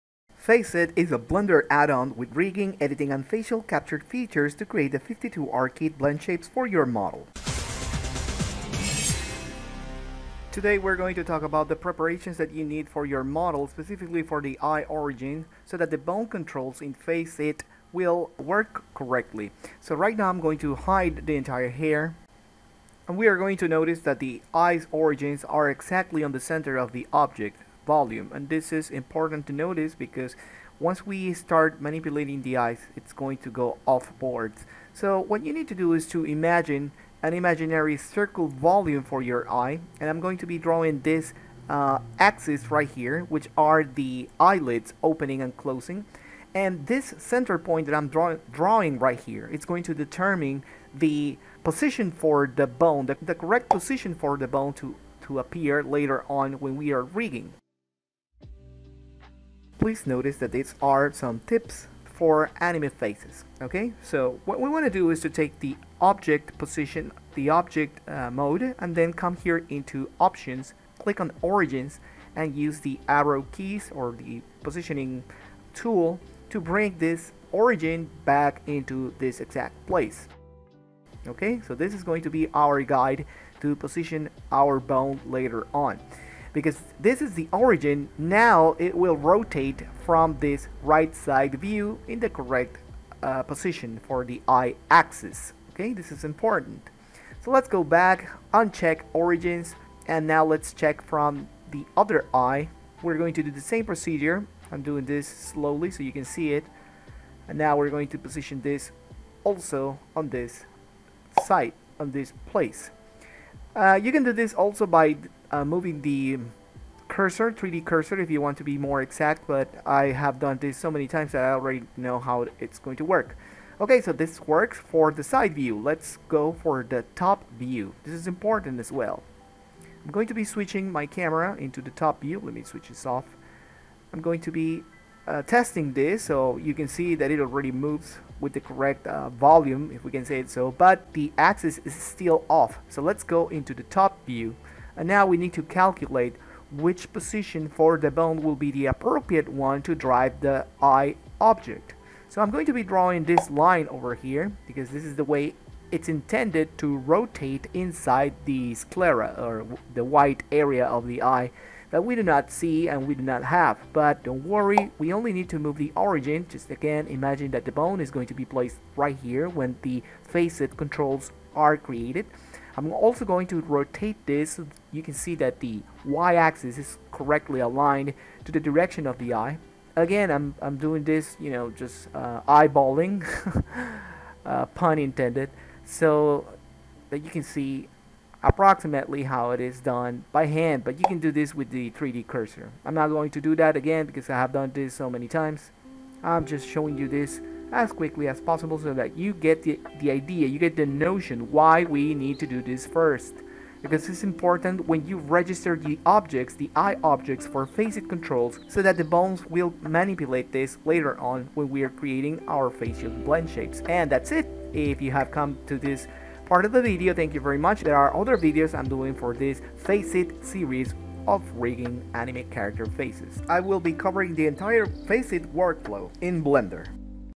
FaceIT addon (Eye origin for Bone controls)
This video will show you how you position your object's origin to correctly register the eye geometry in Anime models for FaceIT using Blender.

I will be doing a complete series covering all aspects of FaceIT. It's an amazing addon. Thank you for your support!